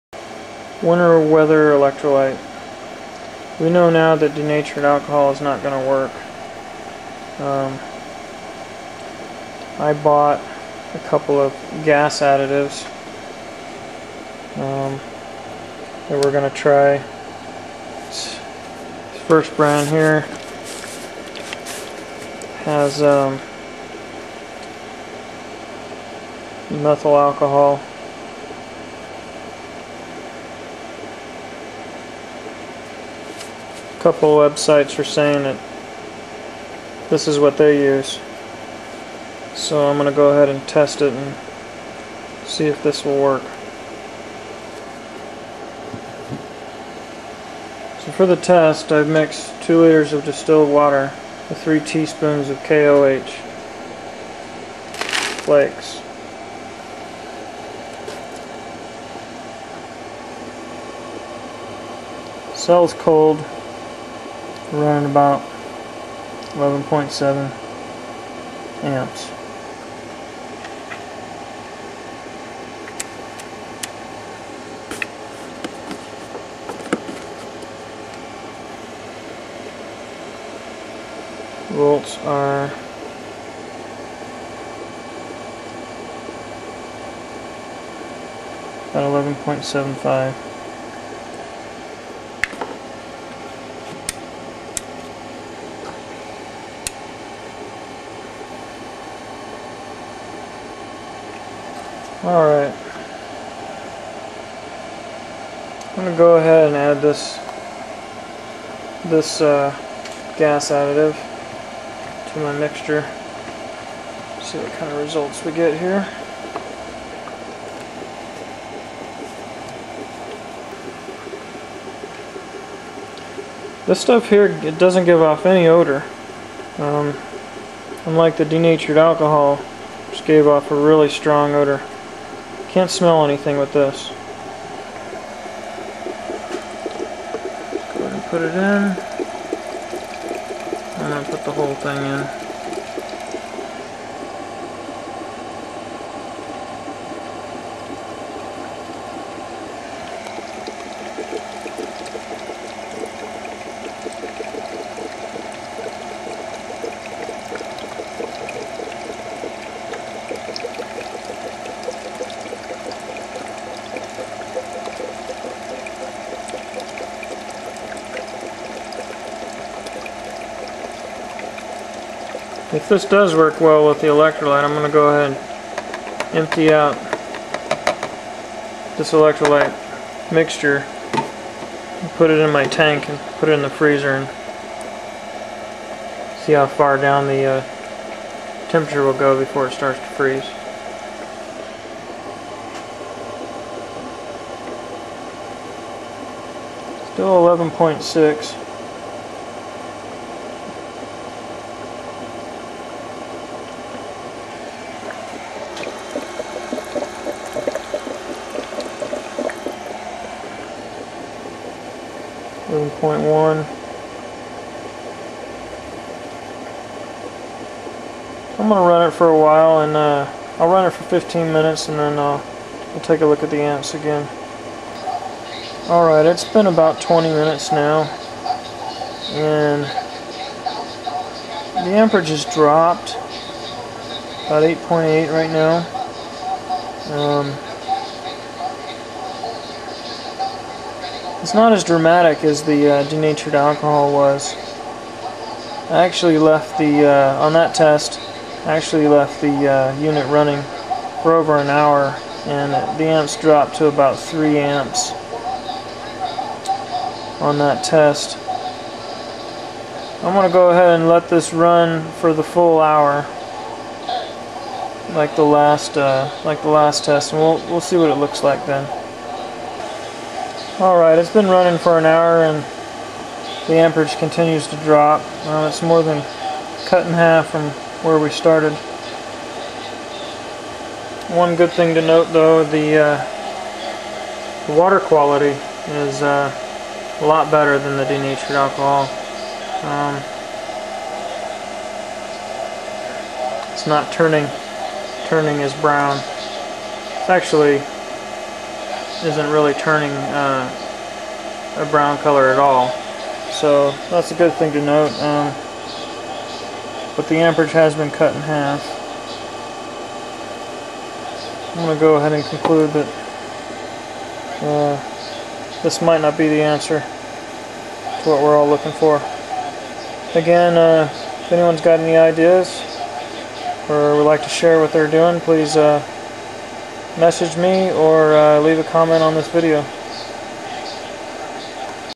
HHO hydrogen Winter Electrolyte? HEET gas treatment?
we are testing here heet brand gas treatment to see if it will keep the water from freezing.. a couple of websites are saying this works. does anyone have an answer?